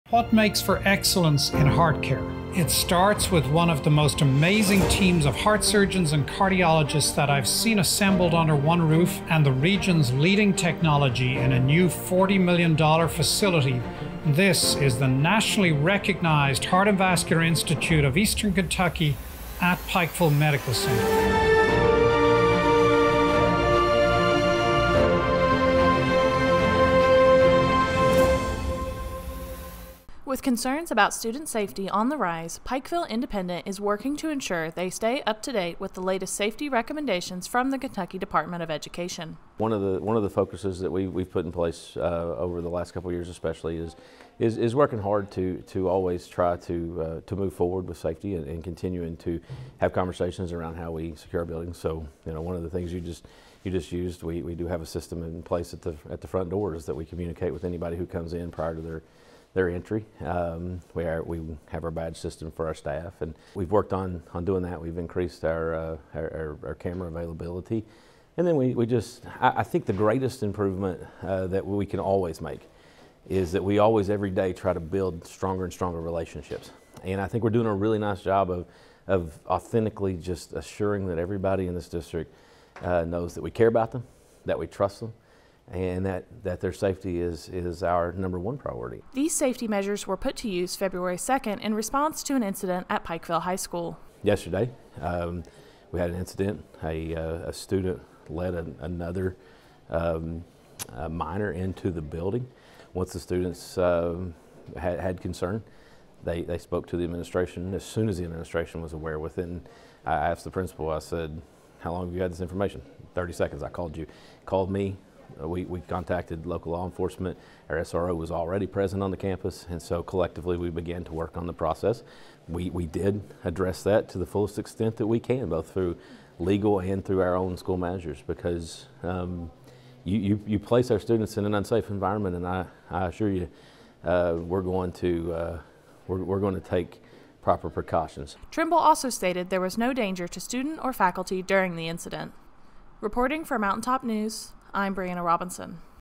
How Pikeville Independent is Staying Up-to-Date on Safety
With concerns over safety on the rise, Pikeville High School has been taking steps to ensure students, faculty, and staff remain safe.